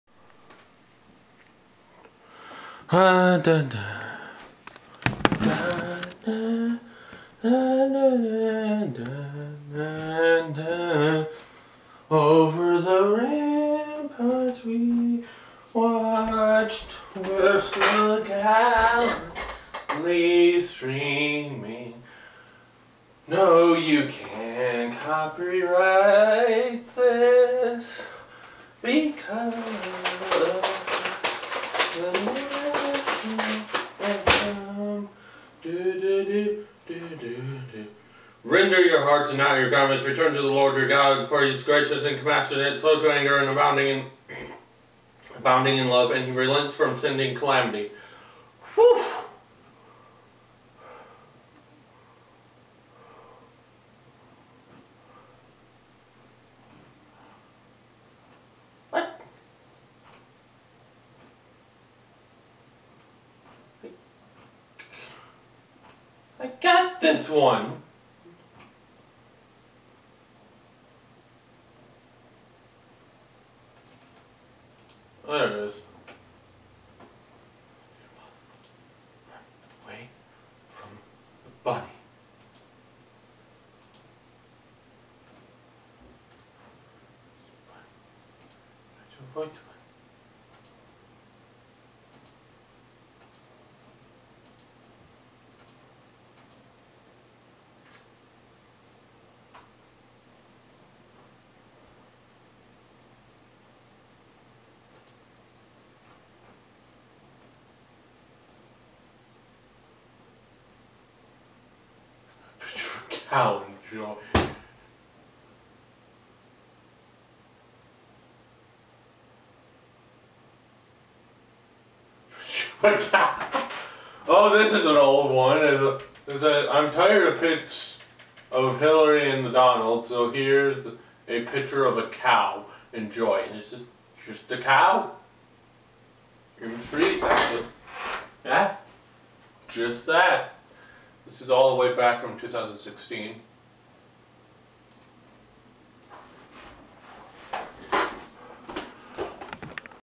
RVOTD 396
Return to the LORD your God, for he is gracious and compassionate, slow to anger and abounding in love, and relents from sending calamity. JOEL 2:13